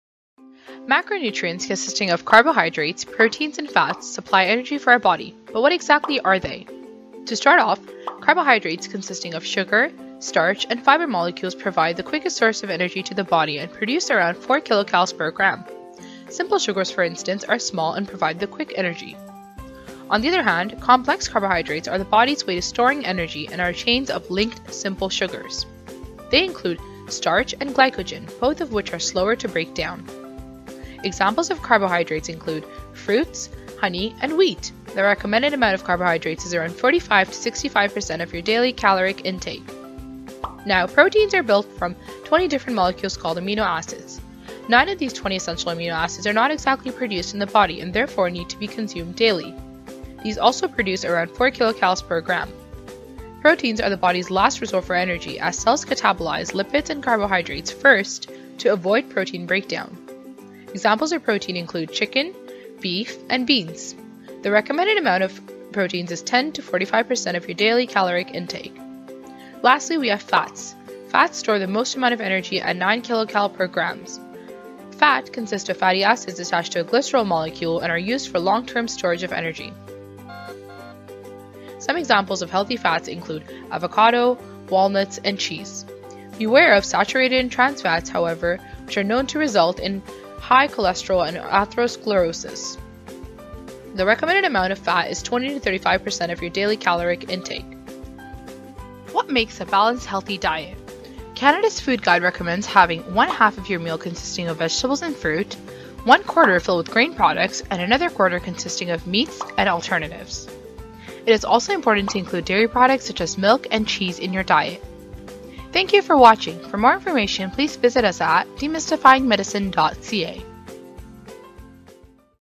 What you need to know about macronutrients
Have you ever wondered what macronutrients are, and why you need them so badly? Is there really a difference in eating fats compared to proteins or carbohydrates? Have you ever wondered you can do to make sure you are eating a balanced meal? Well, you're in luck! Watch this short video to learn more about macronutrients, and what you can do to ensure you are eating the recommended amount for your healthy diet!

This video was created by McMaster Demystifying Medicine students Shara Chowdhury, Coomal Rashid, Ikra Saeed and Manu Sharma

References:

1. Pincivero, D. (2015). Lecture:
Carbohydrates. [PowerPoint presentation]. Retrieved from Avenue to Learn

2. Stanfield, C. L., Germann, W. J., Niles, M. J., & Cannon, J. G. (2011). Principles of human physiology. Pearson/Benjamin Cummings.

3. Pincivero, D. (2015). Lecture:
Nutrition Introduction. [PowerPoint presentation]. Retrieved from Avenue to Learn

4. Pincivero, D. (2015). Lecture:
Proteins. [PowerPoint presentation]. Retrieved from Avenue to Learn

5. Pincivero, D. (2015). Lecture:
Lipids. [PowerPoint presentation]. Retrieved from Avenue to Learn

6. Government of Canada. (n.d.). Build a healthy meal: use the Eat Well Plate.

7. Youdim, A. (n.d.). Carbohydrates, Proteins, and Fats - Disorders of Nutrition - Merck Manuals Consumer Version.